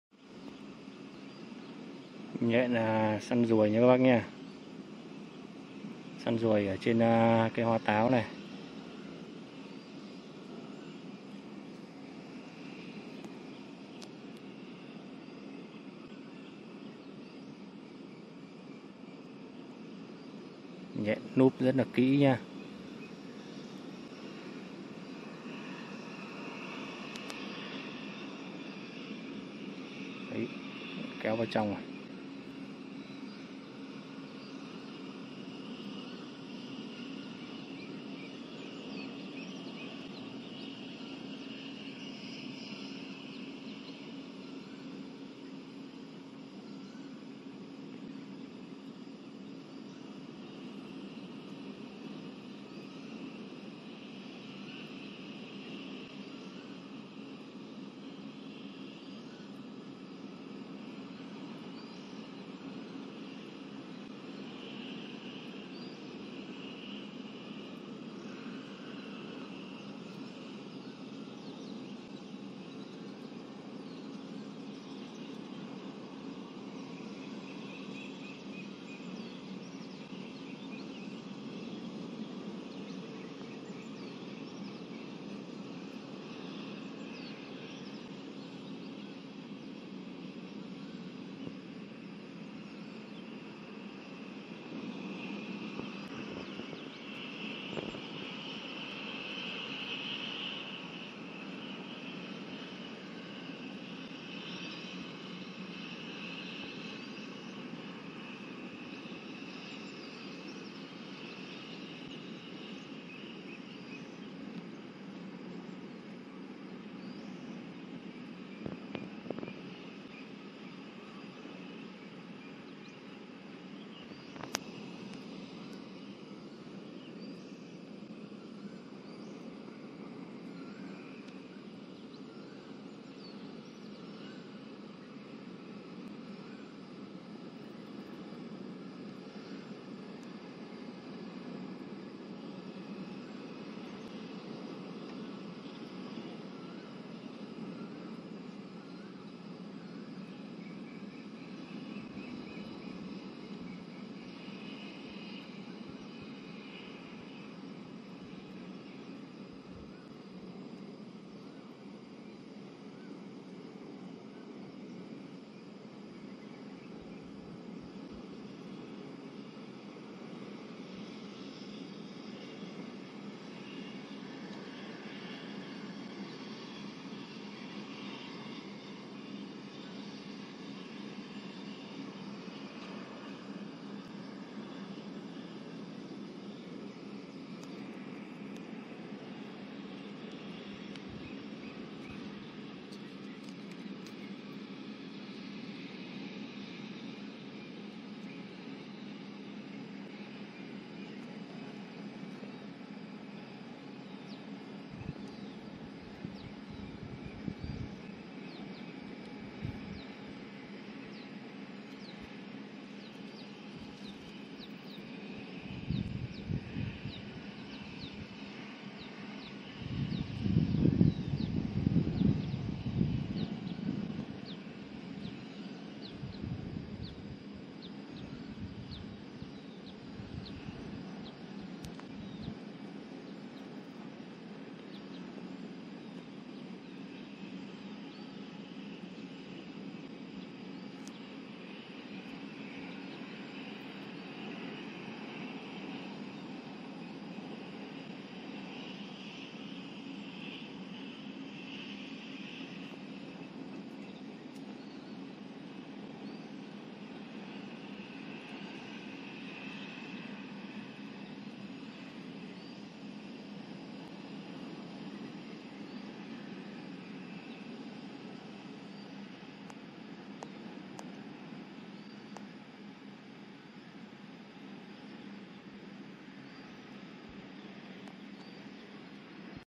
Chú nhện xanh rình bắt ruồi đen trên cành hoa táo | Vô Tranh
chú nhện nguỵ trang và bắt thành công ruồi